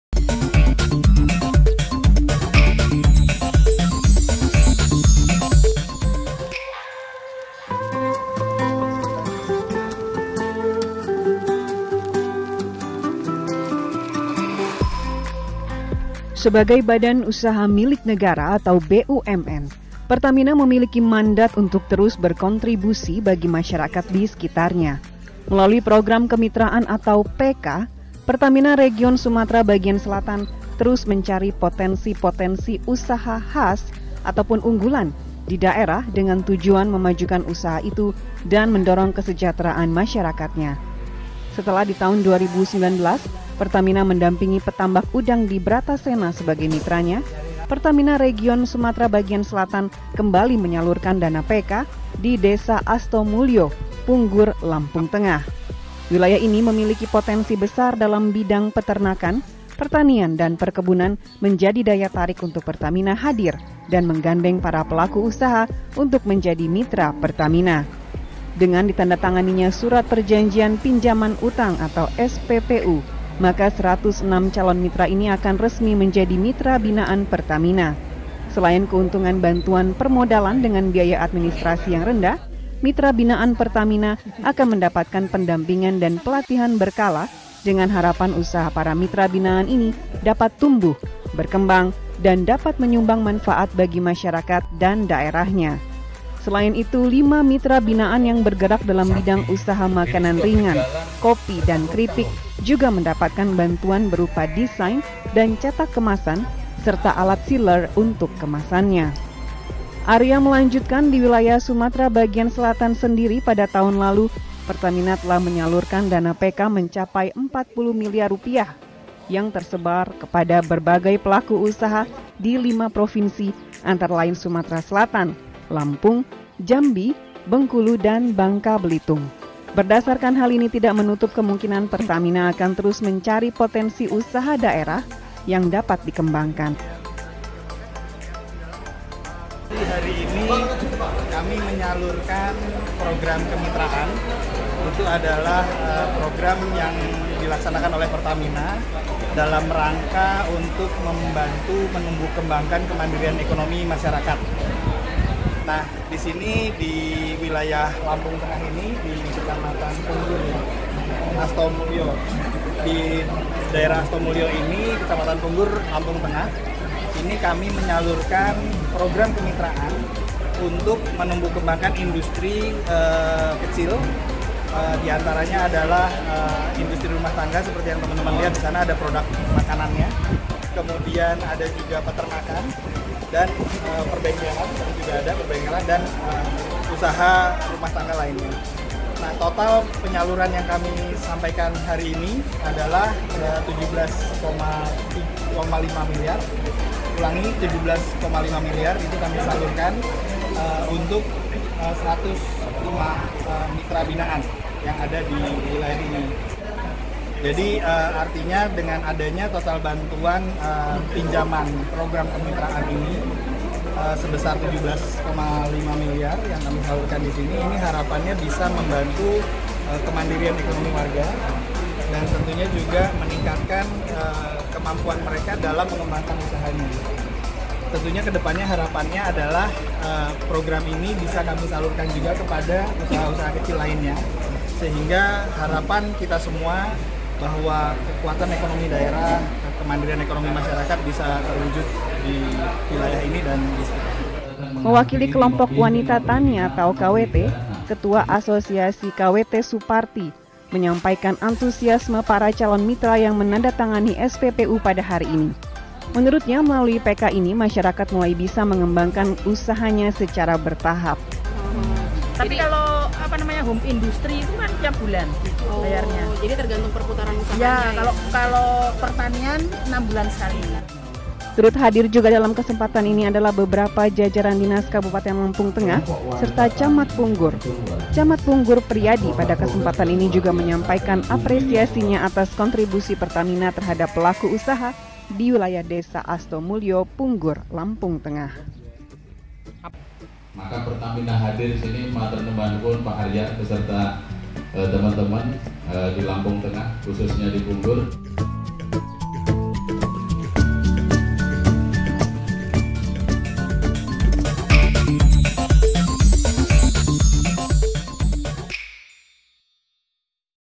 Dorong Kesejahteraan, Pertamina Gandeng Usaha Masyarakat Lampung Tengah
Sebagai Badan Usaha Milik Negara (BUMN), Pertamina memiliki mandat untuk terus berkontribusi bagi masyarakat disekitarnya. Melalui Program Kemitraan (PK), Pertamina Region Sumatera Bagian Selatan terus mencari potensi-potensi usaha khas ataupun unggulan di daerah dengan tujuan memajukan usaha tersebut dan mendorong kesejahteraan masyarakatnya.
Setelah di tahun 2019 Pertamina mendampingi petambak udang di Bratasena sebagai mitranya, Pertamina Region Sumbagsel kembali menyalurkan dana PK di Desa Astomulyo, Punggur, Lampung Tengah. Wilayah ini memiliki potensi besar dalam bidang peternakan, pertanian, dan perkebunan ini menjadi daya tarik untuk Pertamina hadir dan menggandeng para pelaku usaha untuk menjadi mitra Pertamina.
“Mengawali tahun 2020, Pertamina Region Sumbagsel akan menggandeng 106 calon mitra di wilayah Lampung Tengah dengan bidang usaha antara lain peternak sapi, peternak ayam, took sembako, serta bengkel dengan total penyaluran mencapai 17 Milyar Rupiah. Mudah-mudahan ini menjadi start yang bagus untuk kontribusi berkelanjutan Pertamina terhadap perekonomian masyarakat dan daerah,” kata Vice President (VP) Corporate Social Responsibility & Small Medium Enterprise Partnership Program (CSR & SMEPP) PT Pertamina (Persero), Arya Dwi Paramita.
Dengan ditandatanganinya Surat Perjanjian Pinjaman Uang (SPPU), maka 106 calon mitra ini akan resmi menjadi mitra binaan Pertamina. Selain keuntungan bantuan permodalan dengan biaya administrasi yang rendah, mitra binaan Pertamina akan mendapatkan pendampingan dan pelatihan berkala dengan harapan usaha para mitra binaan ini dapat tumbuh, berkembang, dan dapat menyumbang manfaat bagi masyarakat dan daerahnya. Selain itu, lima mitra binaan yang bergerak dalam bidang usaha makanan ringan, kopi, dan keripik juga mendapatkan bantuan berupa desain dan cetak kemasan, serta alat sealer untuk kemasannya.

“Semoga bantuan permodalan, pelatihan, pendampingan, serta bentuk dukungan lainnya dari Pertamina dapat dimanfaatkan dengan baik oleh seluruh mitra binaan. Pertamina dan mitra binaan akan bergandengan mengawal seluruh proses PK ini untuk mencapai tujuan besar tersebut,” ucapnya, Selasa (03/03/2020).
Arya melanjutkan, di wilayah Sumbagsel sendiri pada tahun lalu Pertamina telah menyalurkan dana PK mencapai 40 Milyar Rupiah yang tersebar kepada berbagai pelaku usaha di lima (5) provinsi antara lain Sumatera Selatan, Lampung, Jambi, Bengkulu, dan Bangka Belitung. Berdasarkan hal ini, tidak menutup kemungkinan Pertamina akan terus mencari potensi usaha daerah dapat dikembangkan.
“Tim Pertamina akan menyebar mencari potensi-potensi yang dapat digali dari setiap daerahnya. Kami juga biasanya akan bekerjasama dengan dinas atau kelompok supaya bantuan PK ini dapat tepat sasaran diterima oleh pelaku usaha yang membutuhkan,” jelas Arya.
Mewakili Kelompok Wanita Tani (KWT), Ketua Asosiasi KWT, Suparti menyampaikan antusiasme para calon mitra yang menandatangani SPPU pada hari ini. Menurutnya, melalui PK ini, masyarakat mulai bisa mengembangkan usahanya secara bertahap.
“Mereka adalah pekerja keras. Dengan adanya bantuan permodalan PK ini saya rasa seluruh pelaku usaha ini bisa memulai prioritisasi pengembangan usahanya, dengan demikian usaha mereka akan lebih maju dan lebih bermanfaat lagi,” kata Suparti.
Turut hadir juga dalam kesempatan ini adalah beberapa jajaran Dinas Kabupaten Lampung Tengah serta Camat Punggur. Camat Punggur, Priyadi pada kesempatan ini juga menyampaikan apresiasinya atas kontribusi Pertamina terhadap pelaku usaha di wilayah Desa Astomulyo, Punggur.
“Dengan Program Kemitraan ini, harapannya mereka dapat memberikan kontribusi tambahan terhadap perekonomian daerah. Tidak lupa para mitra ini juga akan menjadi wajah dari hasil daerah khas Punggur, Lampung Tengah,” kata Priyadi dalam sambutannya.***